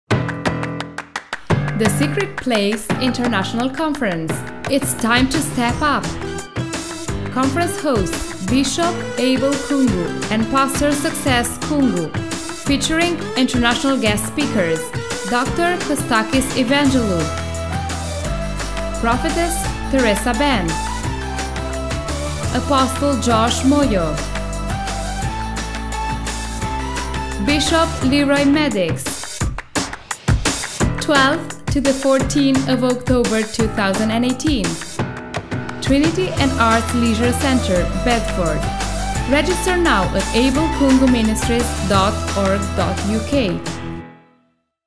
The Secret Place International Conference 2018
1 GATHERING 1 CONFERENCE 3 IMPACTFUL DAYS of powerful WORD, PRAYER and MIRACLES. You need to get here with your Family, Friends and your church. This is your appointed time to STEP UP to your purpose, Dream, Ministry, business etc. See you there!